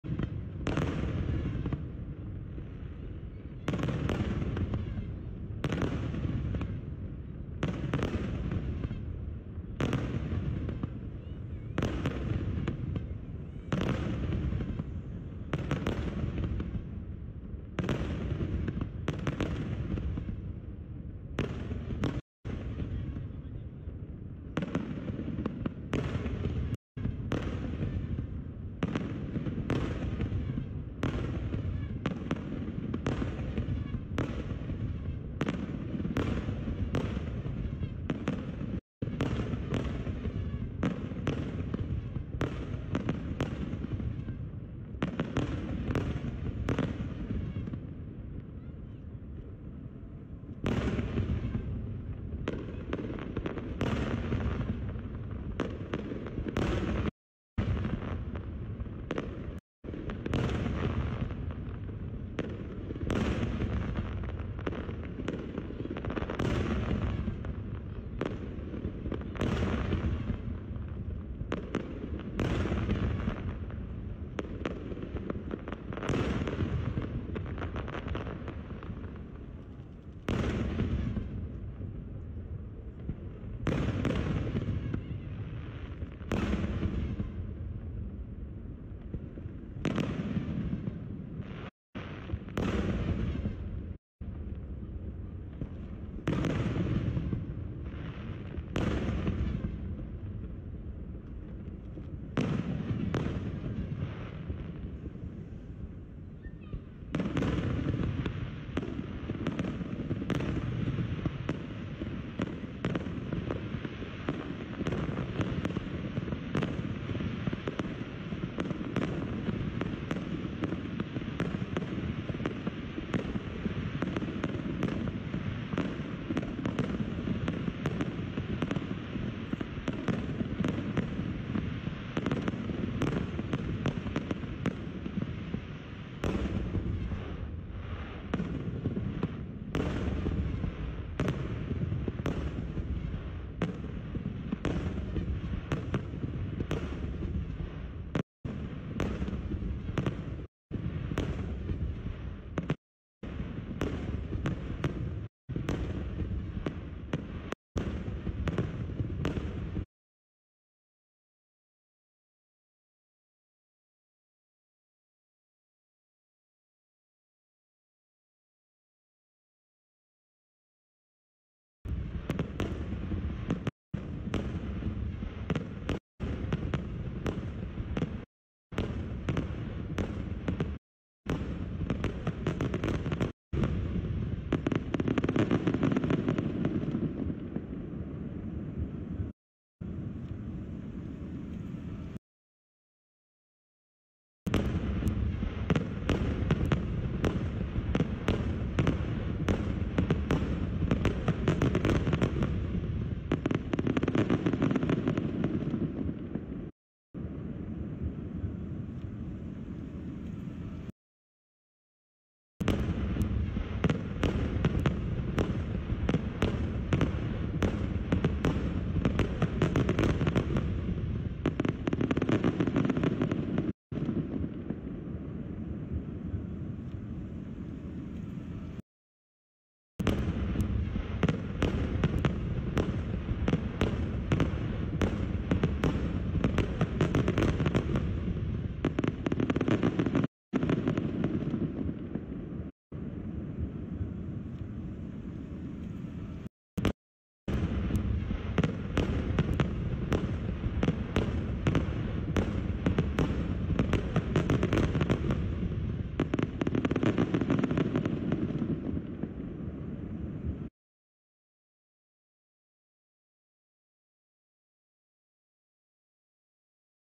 49th UAE NATIONAL DAY FIREWORKS - YAS ISLAND
49th UAE NATIONAL DAY FIREWORKS - YAS ISLAND
ABU DHABI UAE
02/December/2020
02/December/2020
49th UAE  NATIONAL DAY LIVE
Abu Dhabi: A set of fireworks display will light up the night sky on Wednesday in Abu Dhabi and residents and visitors will be able to enjoy the sights and sounds live on television and through social media